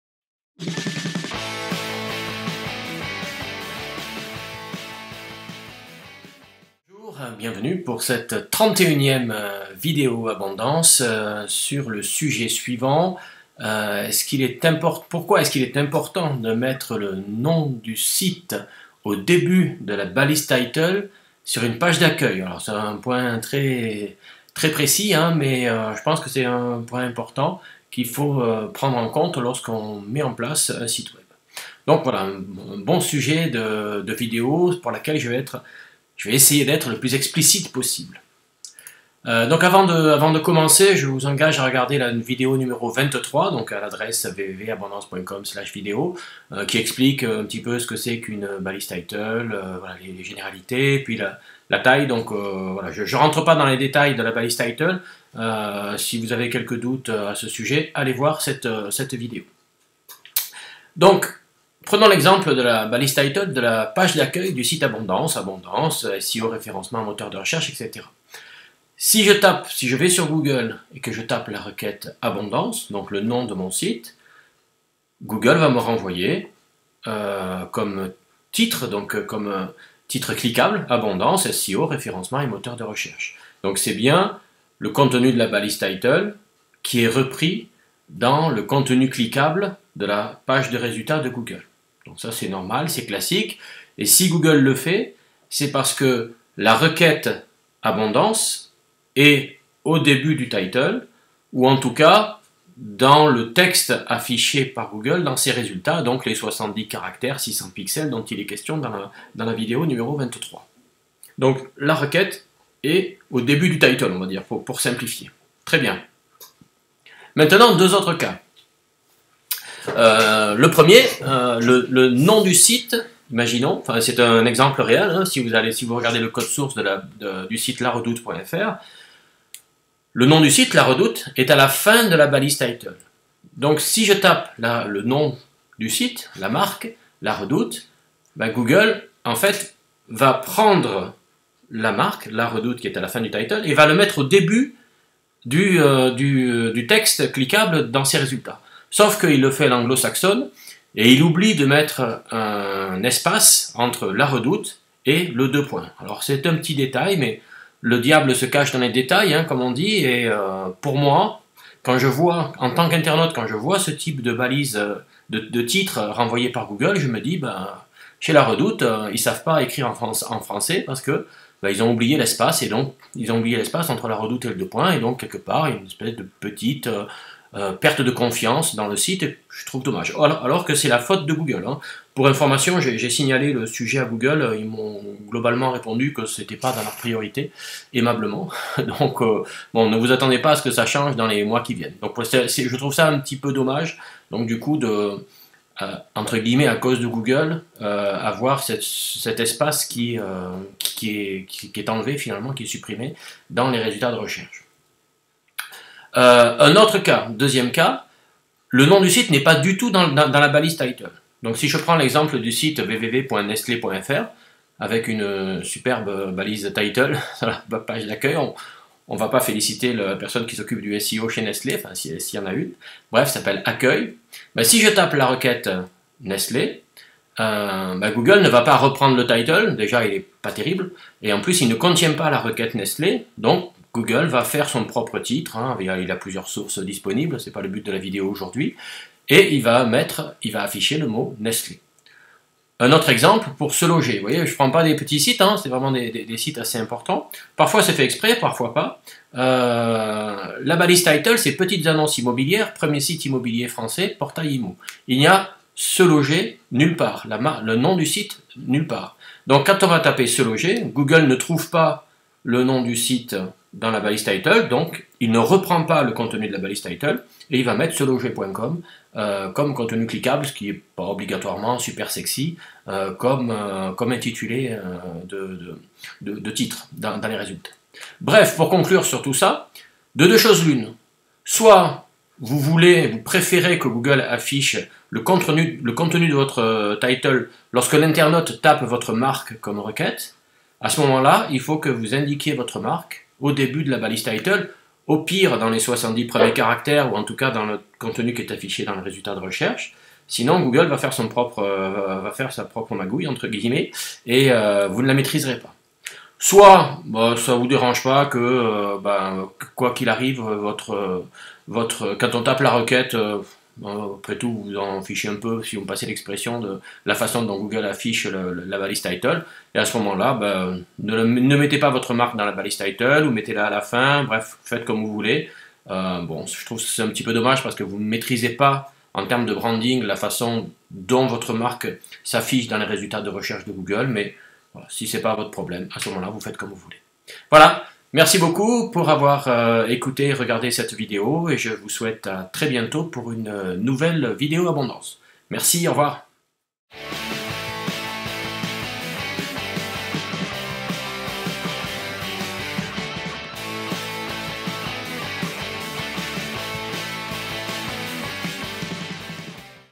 Pourquoi faut-il indiquer le nom du site au début de la balise Title ? Vidéo SEO Abondance
La balise Title est normalement reprise par Google dans ses résultats comme titre cliquable. Mais, lorsque l'internaute tape votre marque comme requête, le moteur peut renvoyer plusieurs résultats différents selon la présence et place de cette information dans votre balise. Vidéo Abondance sur le SEO (référencement naturel) et les moteurs de recherche (Google, Bing, etc.). Retrouvez toutes nos vidéos SEO